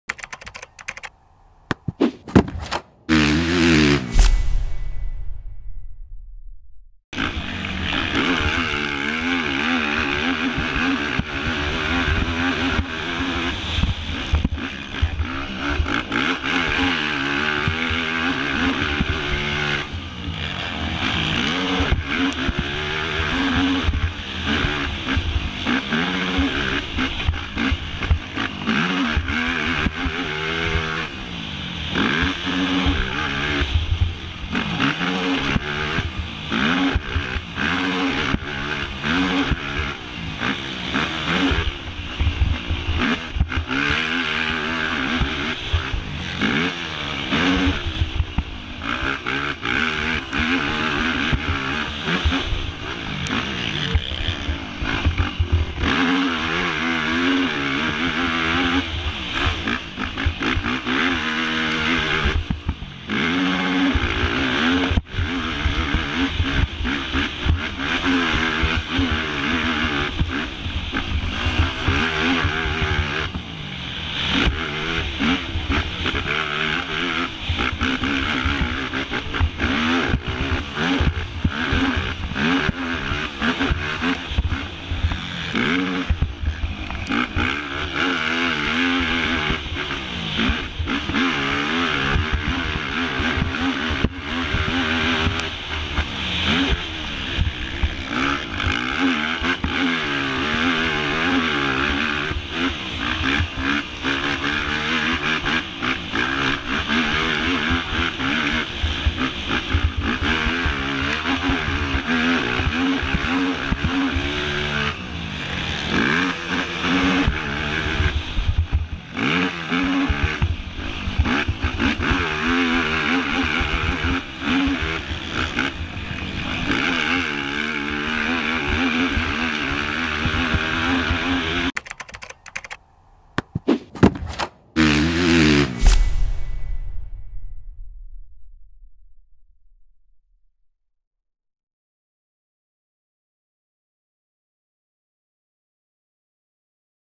MotoSport.com Helmet Cam: Thomas Brown
Hop on board with Thomas Brown as he shows how fast you need to go to compete at the top level in ATV racing.